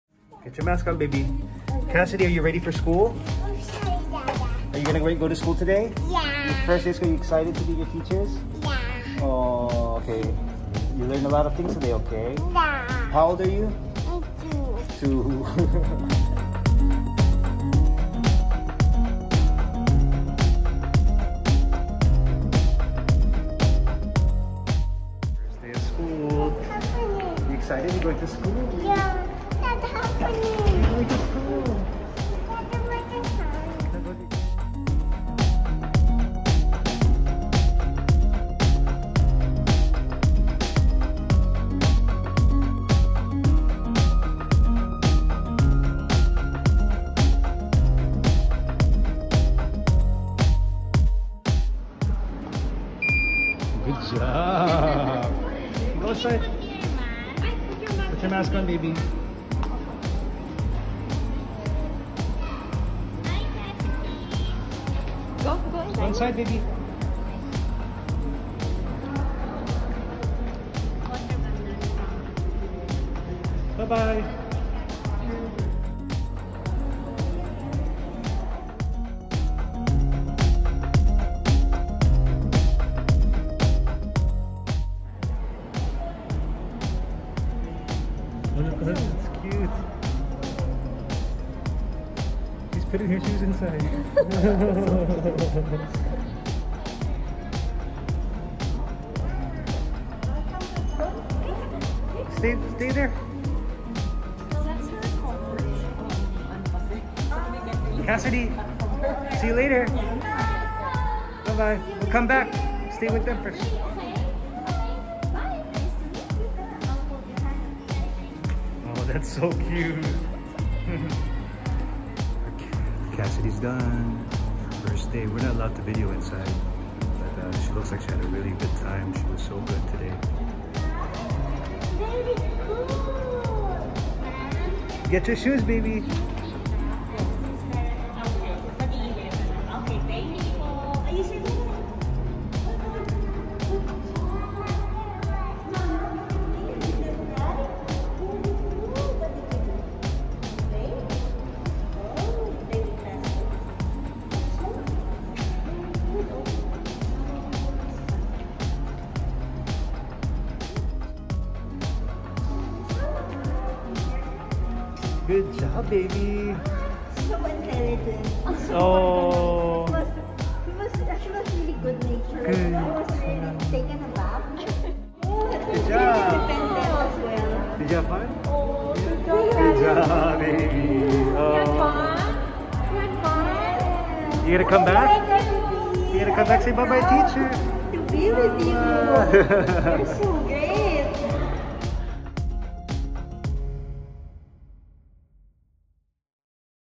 Cassidy’s First Day at Little Clarion
Our little 2 year old girl did her first day at nursery school last week. The teachers were very impressed with how intelligent and kind she was. My wife and I are very proud of her.